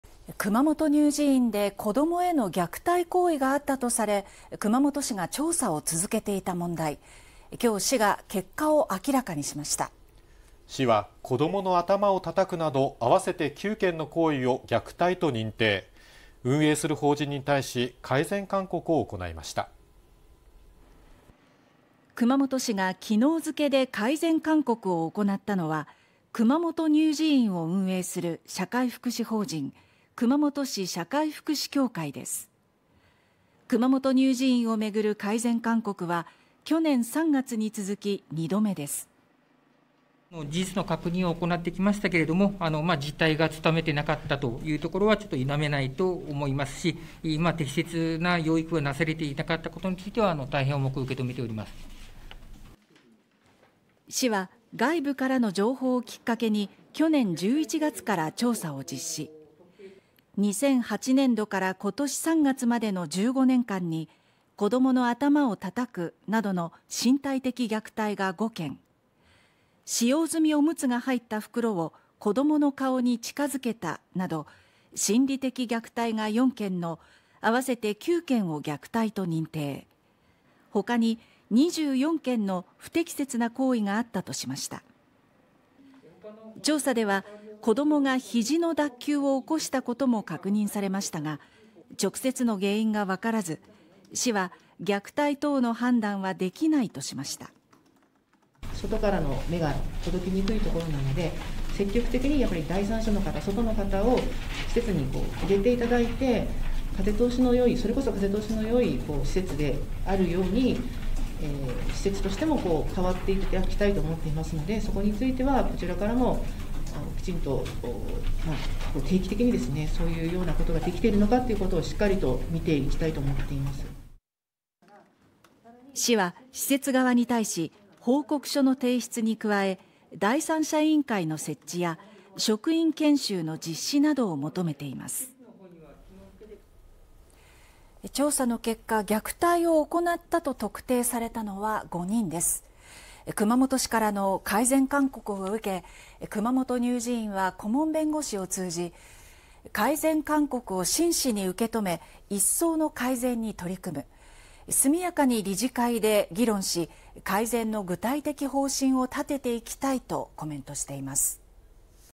熊本乳児院 計９件の行為を虐待と認定、運営する法人に対し改善勧告【熊本】 (23/05/09 18:30)
熊本乳児院で子どもへの虐待行為があったとされ熊本市が調査を続けていた問題、９日、熊本市が結果を明らかにしました。市は「子どもの頭を叩く」など合わせて９件の行為を虐待と認定、運営する法人に対し改善勧告を行いました。

熊本市が８日付けで改善勧告を行ったのは熊本乳児院を運営する社会福祉法人・熊本市社会福祉協会です。

熊本乳児院をめぐる改善勧告は去年３月に続き２度目です。

【熊本市こども家庭福祉課　西嶋 達也 課長】
「事実確認してきたが実態がつかめなかったことは否めない。大変重く受け止めている」

市は外部からの情報をきっかけに去年１１月から調査を実施。

２００８年度から今年３月までの１５年間に「頭を叩く」などの身体的虐待が５件、「使用済オムツが入った袋を子どもの顔に近づけた」など心理的虐待が４件の
合わせて９件を虐待と認定、ほかに２４件の不適切な行為があったとしました。

調査では「こどもが肘の脱臼を起こした」ことも確認されましたが直接の原因がわからず、市は「虐待等の判断はできない」としました。

【熊本市  光安 一美 こども福祉部長】
「外からの目が届きにくいので外の人を施設に入れて風通しのよい施設に変わってほしい」

市は施設側に対し報告書の提出に加え、第三者委員会の設置や職員研修実施などを求めています。

調査によると、虐待を行ったのは５人です。

熊本市からの改善勧告を受け熊本乳児院は、顧問弁護士を通じ「改善勧告を真摯に受け止め、一層の改善に取り組む。速やかに理事会で議論し、改善の具体的方針を立てていきたい」とコメントしています。